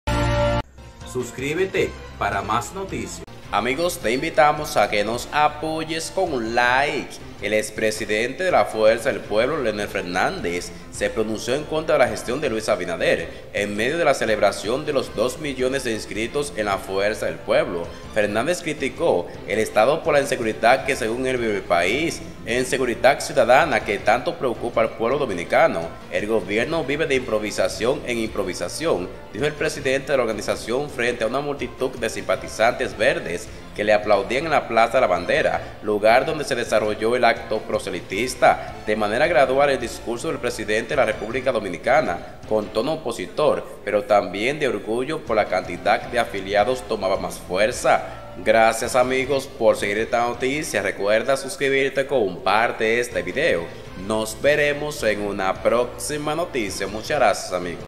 LO QUE ASEGURÓ LEONEL FERNÁNDEZ QUE VIVE LUIS ABINADER
El presidente de la Fuerza del Pueblo (FP), Leonel Fernández, se ha pronunciado en contra de la gestión de Luis Abinader.

En medio de una celebración, que algunos interpretaron como un acto proselitista, con motivo a los más de dos millones de militantes inscritos en la FP, Fernández criticó al Estado por la “inseguridad” que, según él, vive el país.

“En seguridad ciudadana, qué tanto preocupa al pueblo dominicano, el Gobierno vive de improvisación en improvisación”, dijo el presidente de la organización frente a una multitud de simpatizantes verdes que le aplaudían en la Plaza de la Bandera, lugar donde se desarrolló el acto político.

Sobre la reunión semanal del presidente Luis Abinader en el Palacio de la Policía, señaló que el mandatario “todos los lunes se convierte en el jefe de la policía” y afirmó que, cada vez, el pueblo dominicano tiene más “temor” y más “aprensión”.